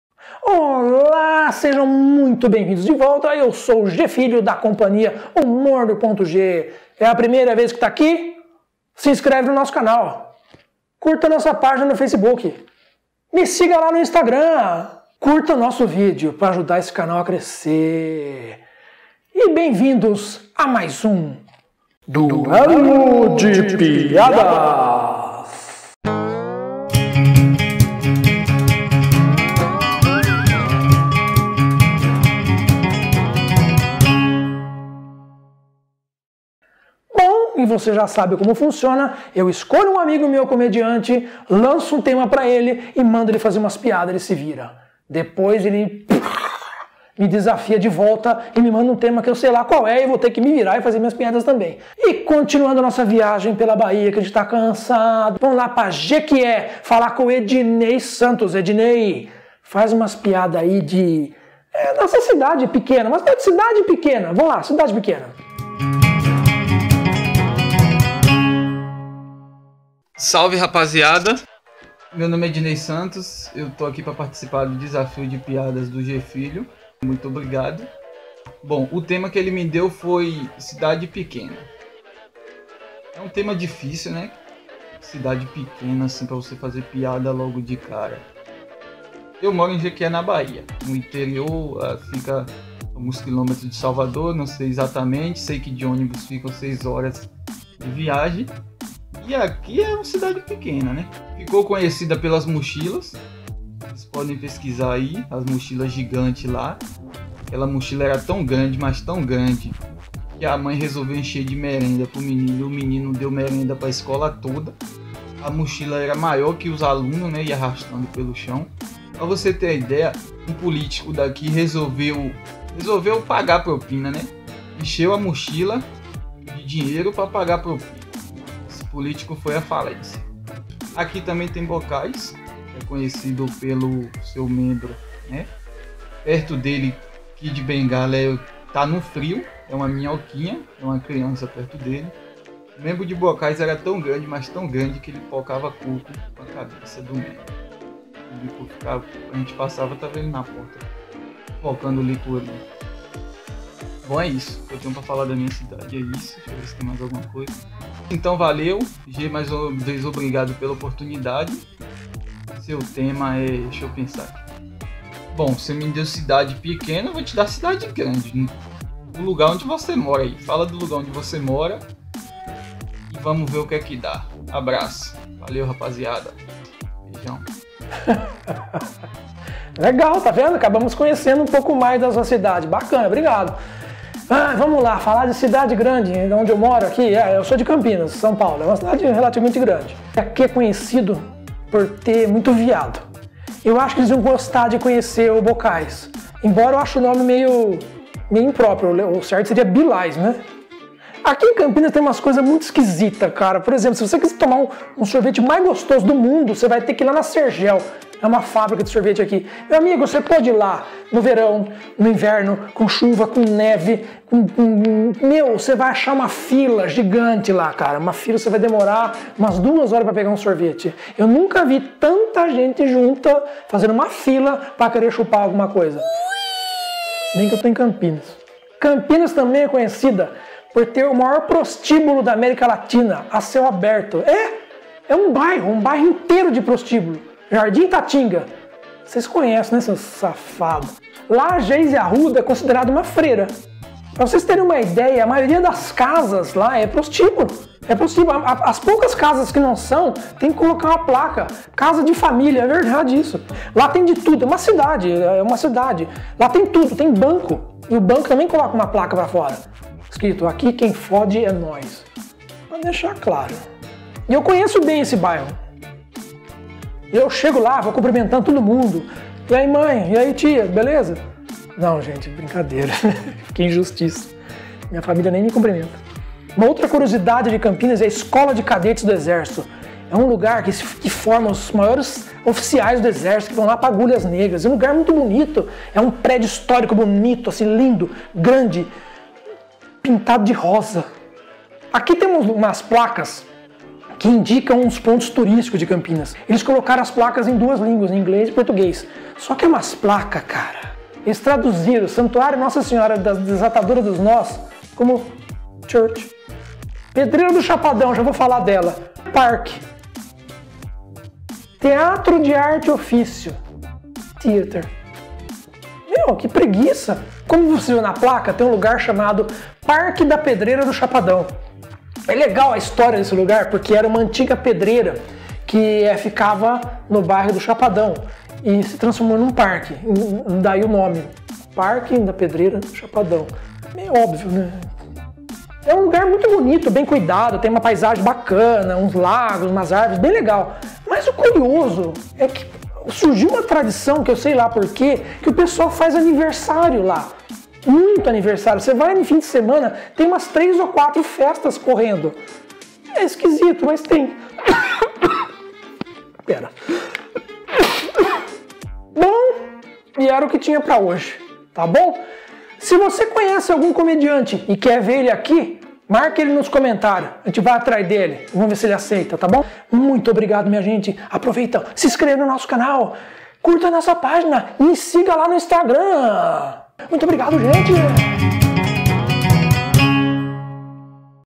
Duelo de Piadas - Edinei Santos x Gê Filho
Uma brincadeiras entre comediantes, eu escolho um tema e ele tem que fazer piadas sobre isso, depois ele me desafia de volta e eu tenho que fazer piadas com o tema que ele propôs !

Convidado : Edinei Santos

Música tema : Diego Ferreira